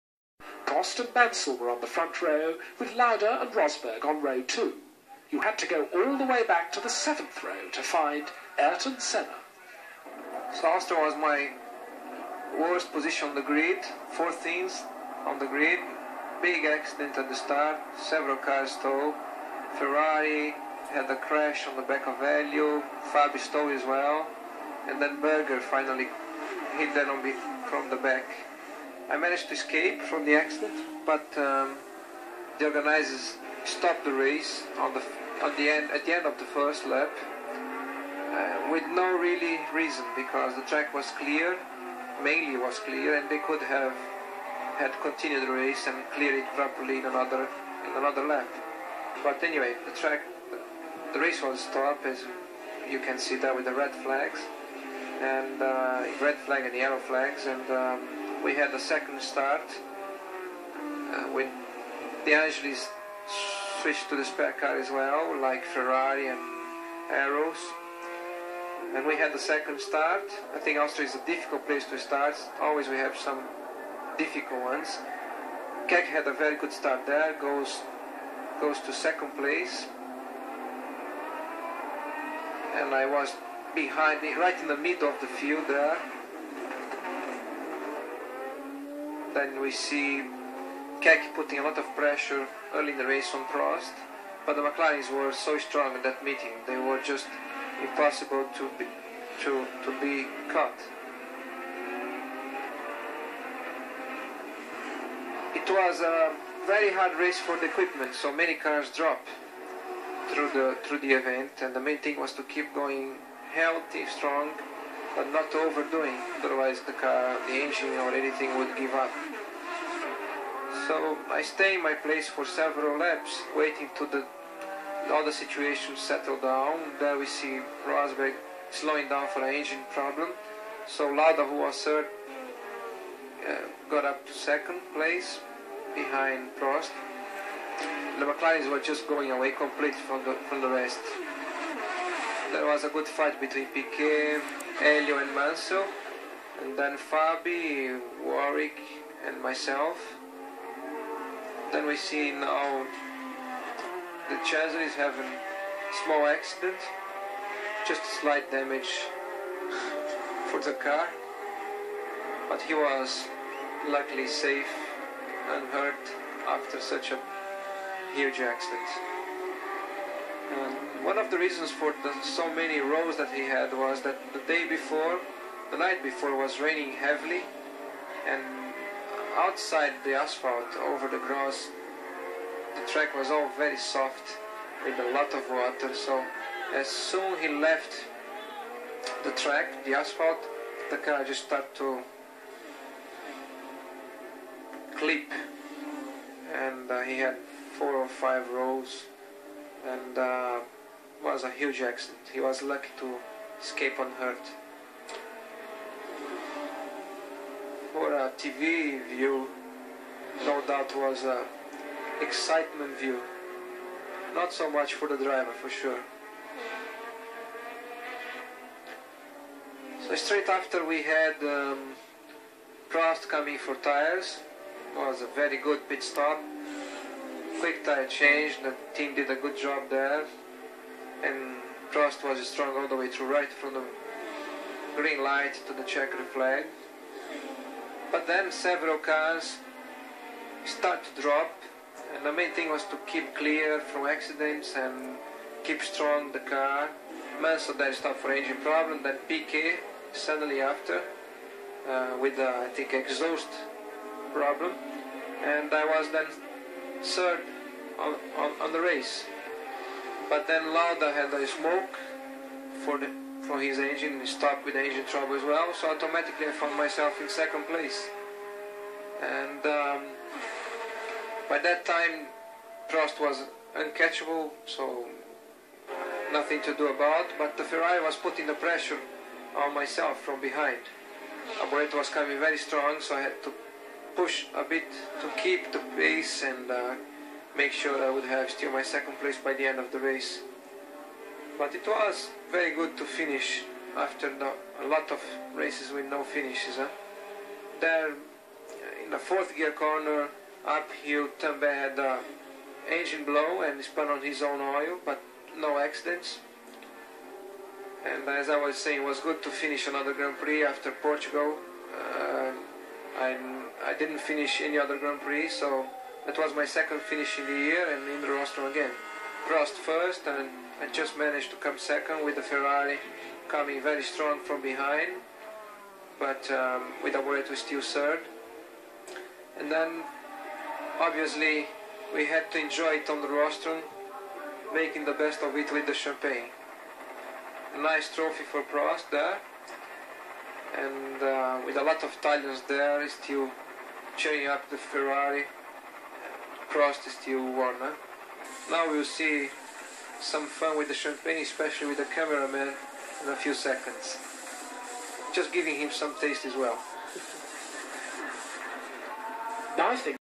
Ayrton Senna Narrando o GP da Áustria 1985
Ayrton Senna narrating the Austrian GP of 1985

VÍDEO RARO: SENNA FALA SOBRE GP DA F-1 EM QUE CHEGOU NO PÓDIO DEPOIS DE LARGAR EM 14º

O Grande Prêmio da Áustria de 1985 contou com uma das performances mais impressionantes de Ayrton Senna na história da F-1. O piloto brasileiro largou da 14ª posição com a Lotus e terminou a corrida em 2º luga na prova  realizada em 1º de julho – que completa hoje 32 anos.

Em um vídeo raro na Internet, Ayrton Senna comentou os melhores momentos dessa corrida e fez uma análise assistindo as imagens da televisão. São seis minutos do piloto brasileiro falando sobre os acontecimentos do GP disputado nos arredores da pequena e bucólica cidade de Spielberg. O brasileiro avaliou o comportamento do seu carro e também o rendimento de seus adversários na traiçoeira pista de Österreichring.  (áudio em inglês – legenda completa logo abaixo do vídeo)

Narrador: Prost e Mansell na primeira fila, com Lauda e Rosberg na segunda fila. Você teria que ir até a sétima fila para encontrar Ayrton Senna.

Ayrton Senna:

“A Áustria foi a minha pior posição no grid. Eu estava em 14º. Houve um grande acidente na largada. Vários carros ficaram parados. A Ferrari bateu na traseira de Elio. Fabi também não se mexeu. E Berger também não conseguiu largar. Eu consegui fugir do acidente, mas os organizadores pararam a corrida no fim da primeira volta, sem nenhum motivo, porque a pista estava limpa. Ela estava basicamente limpa. Eles podiam ter continuado a corrida e limpado direito na volta seguinte. Mas, enfim, a corrida foi parada, como você pode ver aí, com as bandeiras vermelhas – as bandeiras vermelhas e as amarelas. E houve uma segunda largada. De Angelis pegou o carro reserva, assim como a Ferrari e a Arrows. E tivemos a segunda largada. Eu acho a largada na Áustria difícil. A largada é sempre complicada. Keke teve uma ótima largada e pulou para o segundo lugar. Eu estava bem no meio do pelotão, ali. Aí, vemos Keke botando muita pressão sobre Prost no começo da corrida. Mas as McLarens estavam tão fortes nesta etapa que era impossível alcançá-las”.

“Foi uma corrida muito dura para o equipamento, então muitos carros abandonaram ao longo do evento. O principal era manter um ritmo forte e saudável, mas sem exagerar. Senão, o motor ou alguma outra coisa iria falhar. Então, eu mantive a minha posição por várias voltas, esperando a situação se definir. Aqui, vemos Rosberg desacelerando devido a um problema de motor. Então, o Lauda, que estava em terceiro, passou para o segundo lugar, atrás do Prost. As McLarens estavam se separando totalmente dos demais. Houve uma boa briga entre Piquet, Elio e Mansell. Depois, vinham Fabi, Warwick e eu”.

“Agora, vemos o De Cesaris sofrendo um pequeno acidente. O carro sofreu apenas danos leves. Felizmente, ele escapou ileso, depois de um acidente tão grave. Um dos motivos porque ele capotou tantas vezes foi que, na noite anterior, choveu muito. E fora do asfalto, na grama, o piso estava muito macio, com muita água. Então, assim que ele saiu da pista, do asfalto, o carro começou a saltar. E ele capotou quatro ou cinco vezes. Foi um acidente enorme.  Ele teve sorte em escapar ileso. Vendo pela TV, sem dúvida é empolgante. Mas para o piloto, nem tanto, com certeza”.

“Logo depois, o Prost trocou os pneus. Foi um excelente pit-stop. A equipe fez um ótimo trabalho, na troca. E o Prost andou muito forte desde a luz verde (largada) até a bandeira quadriculada. Então, muitos carros começaram a abandonar. O mais importante era escapar dos acidentes e manter um ritmo forte. O Mansell parou com problemas no motor, e o Piquet na sequência – acho que com problemas no exaustor. Então, eu fiquei em terceiro, na corrida”.

“Mas aí saiu fumaça do motor do Lauda e ele também parou com problemas de motor. Automaticamente, eu me vi em segundo lugar”.

“Àquela altura, o Prost era inalcançável. Então, eu não podia fazer nada a respeito. Mas a Ferrari estava me pressionando, logo trás. Alboreto estava vindo com tudo, então eu tive que forçar um pouco para manter o ritmo e garantir o meu segundo lugar no fim da corrida. Mas foi muito bom terminar, depois de várias corridas sem terminar”.

“Aqui, na curva em quarta marcha, na subida, o motor do Tambay explodiu e ele rodou no próprio óleo. Mas não houve acidente”.

“Como eu ia dizendo, foi bom terminar outro GP. Depois de Portugal, eu não tinha terminado mais nenhum GP. Foi a segunda vez em que terminei uma corrida no ano e cheguei no pódio de novo. O Prost chegou em primeiro e eu consegui terminar em segundo com uma Ferrari vindo forte, logo atrás – com o Alboreto ainda em terceiro. Depois, obviamente, nós curtimos o pódio, fazendo a festa com o champanhe. Lindo troféu para o Prost. Havia muitos italianos lá, torcendo pela Ferrari. E o Prost venceu”.

“Agora, você vê a gente se divertindo com o champanhe, especialmente com o cinegrafista, em alguns segundos. Para ele provar um pouquinho”, encerra Senna.